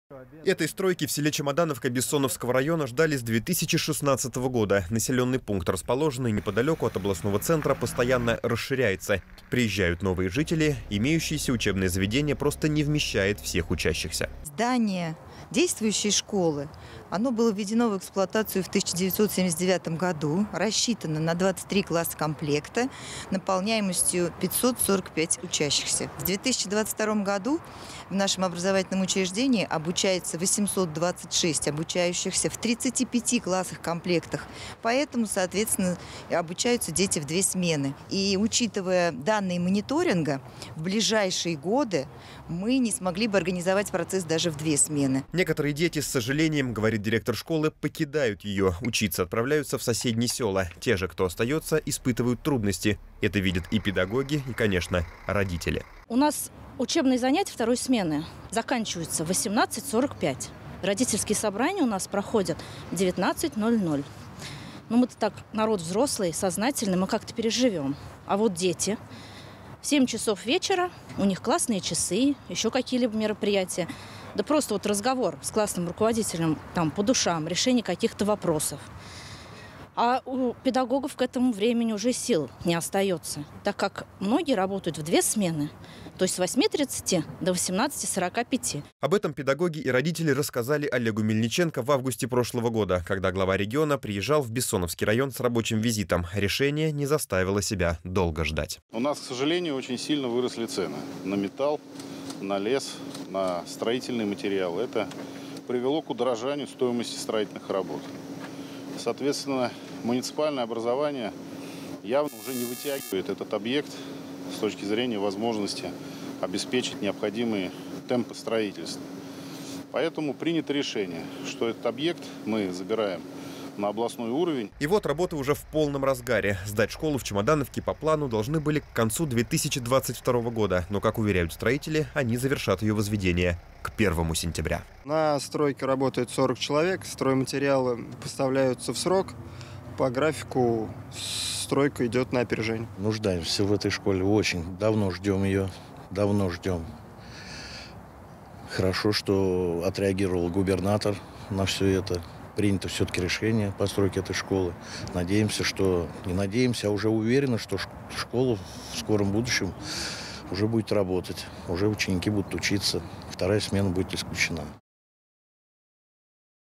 В селе Чемодановка идет активное строительство новой школы
За прошлый год в Пензенской области было отремонтировано 35 школ на общую сумму почти в 110 миллионов рублей. В 2022 году эта работа продолжатся. Кроме того, возводятся новые учебные заведения. Сразу несколько строятся в Пензе. Еще одна школа к первому сентября должна открыться в селе Чемодановка.